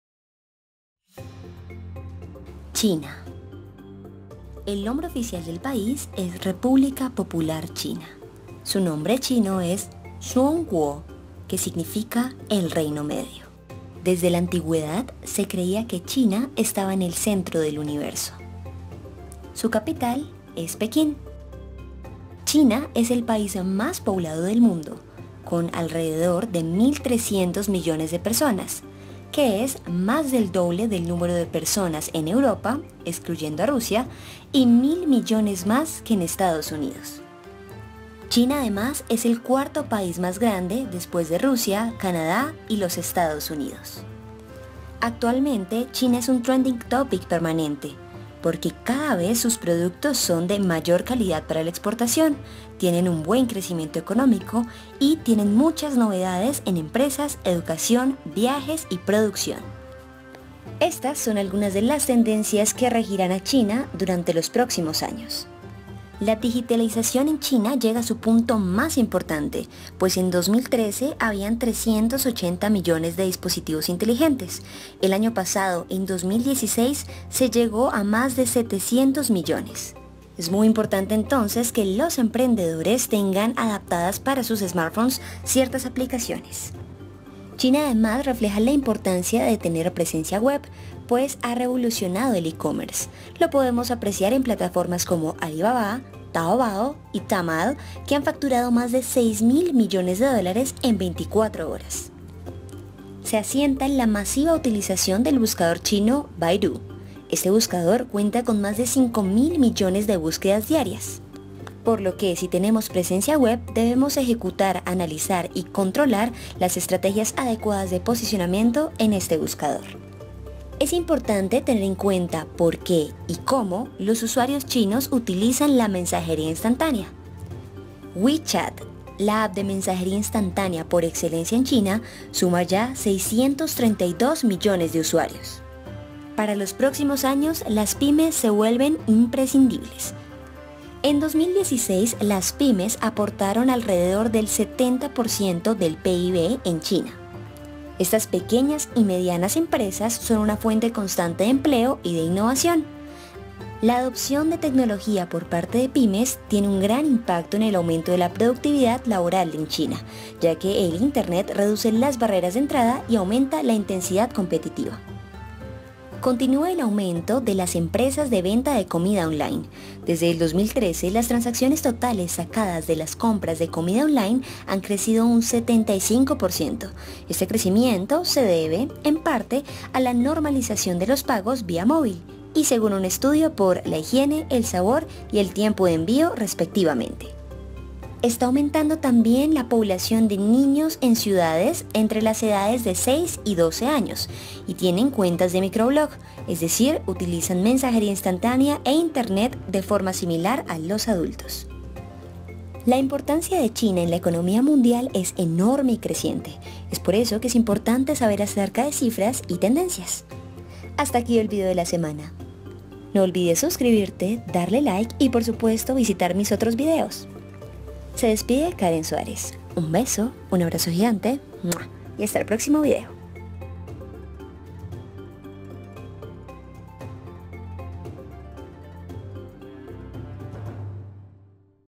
TENDENCIAS CLAVE DE CONSUMO EN CHINA
China es un mercado enorme, importante y creciente. En éste vídeo se explican las tendencias más importantes de la economía china

Karem, con m